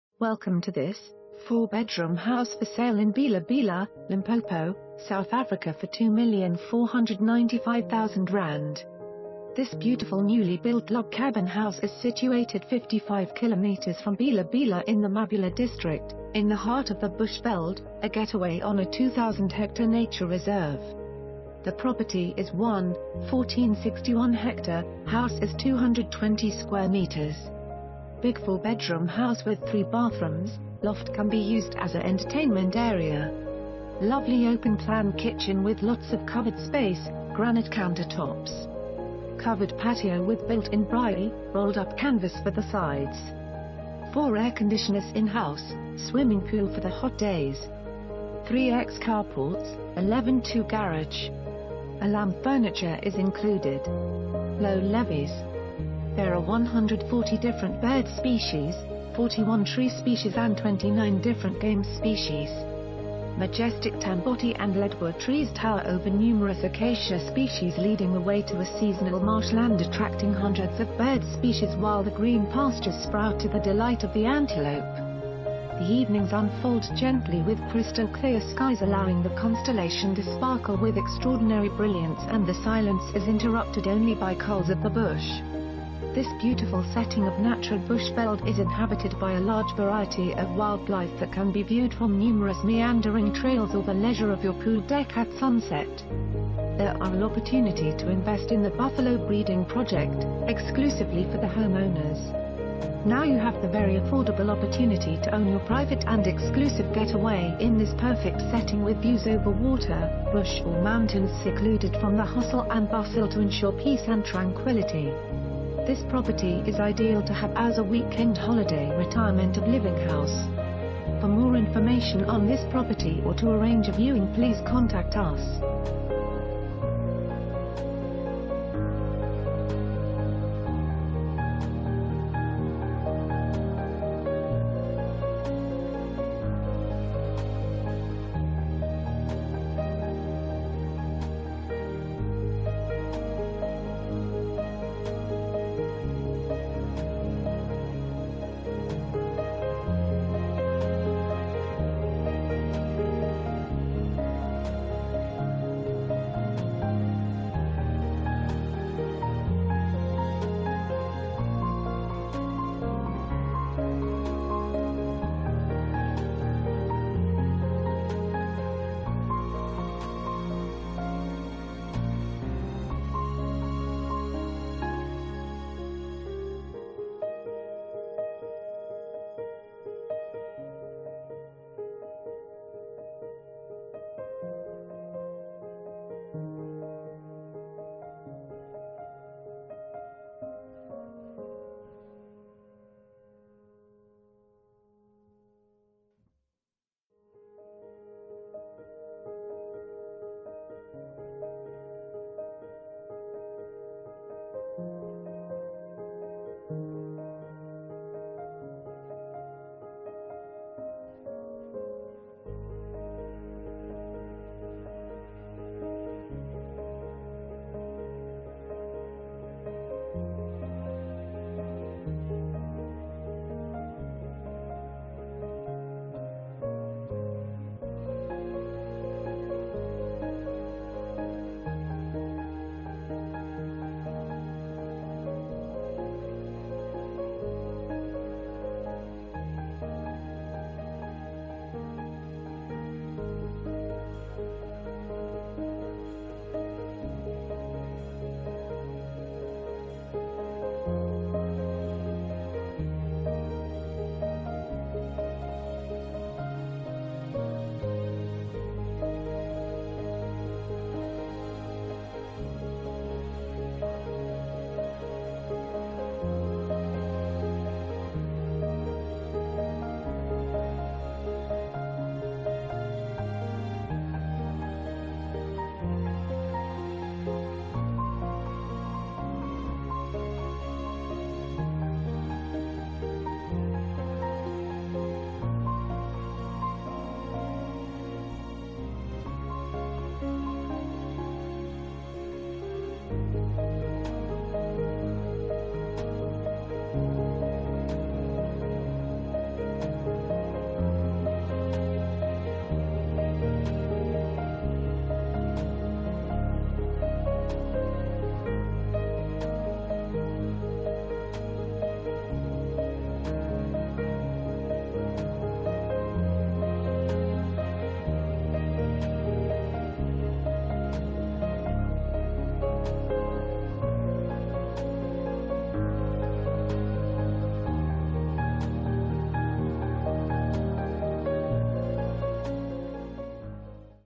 4 Bedroom House For Sale in Bela Bela, Limpopo, South Africa for ZAR 2,495,000
This beautiful newly built log Cabin House is Situated 55km from Bela-Bela in the Mabula district,  in the hart of the BUSHVELD , a GETAWAY ON A 2000HA NATURE RESERVE.  The property is 1,1461ha, house is 220m2.  Big 4 bedroom house with 3 bathrooms, loft can be used as a entertainment area. Lovely open plan kitchen with lots ...